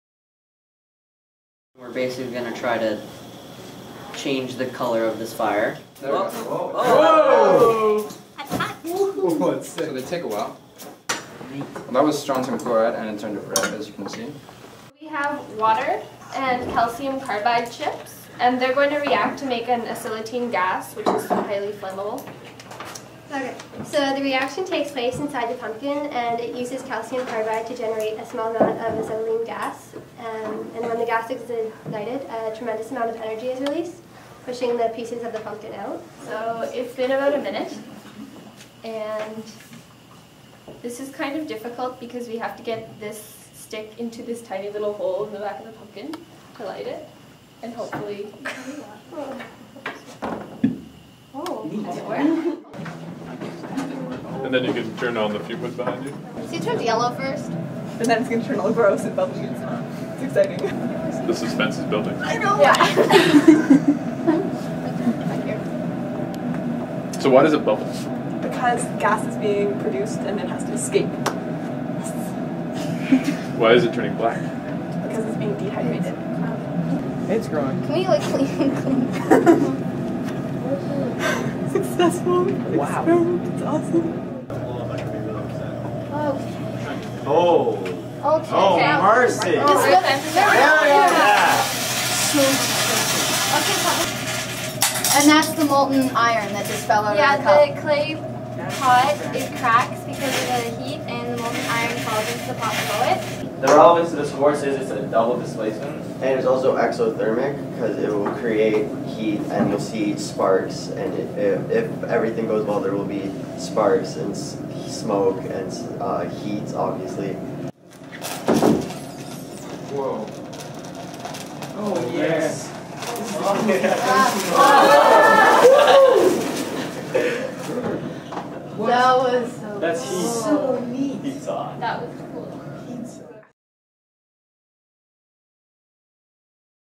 Ridley College Grade 10 Chemistry
Ridley College Private Canadian High School - Grade 10 Chemistry Class demonstrations